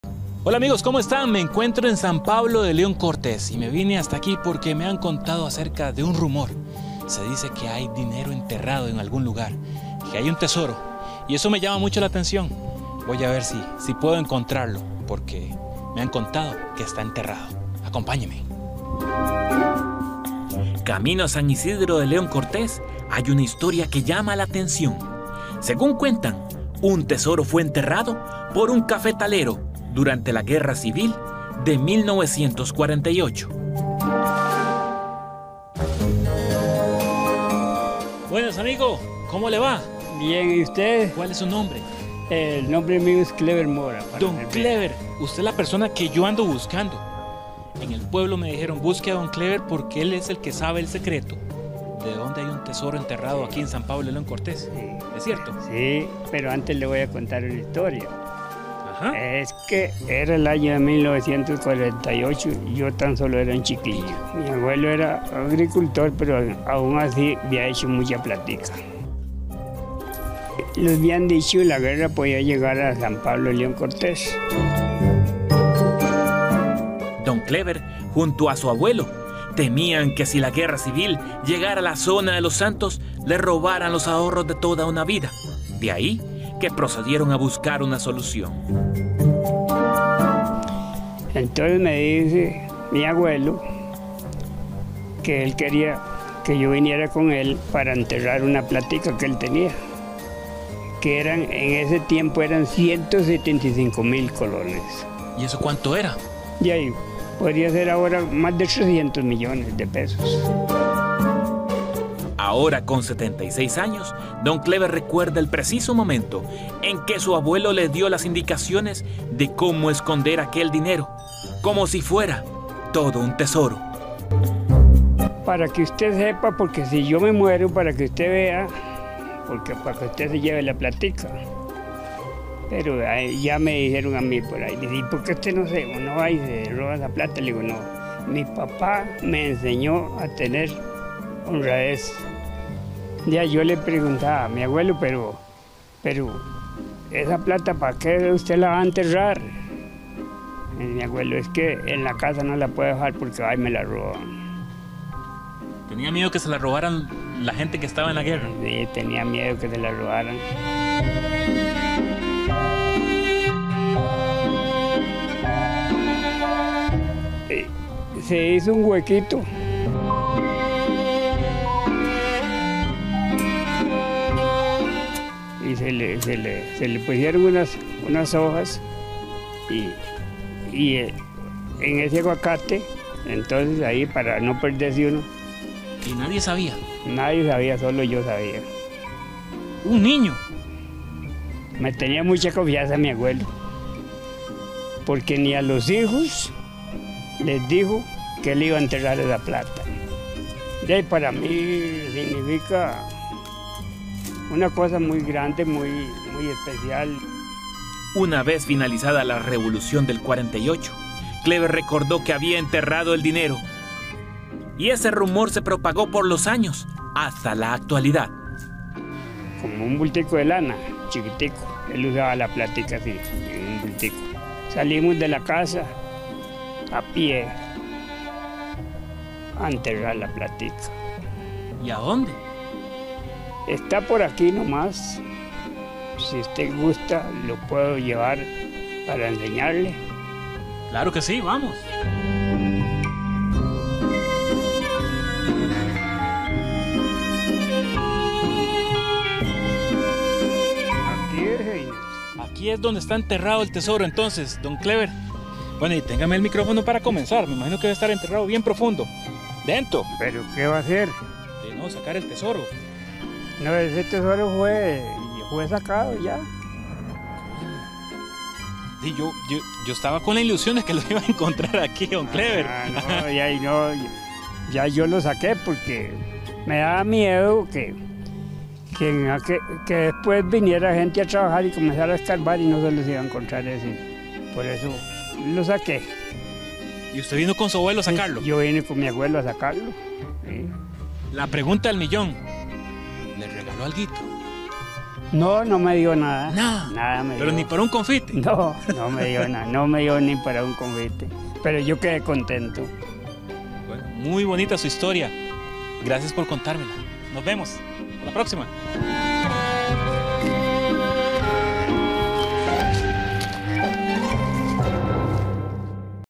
¡Fuimos a buscar un tesoro escondido en la zona de Los Santos!
¿Será que logra encontrar algo?

Cuenta una historia que en un cafetal de la zona de Los Santos hay un tesoro escondido. Es por esta razón que nuestro compañero Heiner Salazar fue a ver si era cierto porque dicen que está enterrado.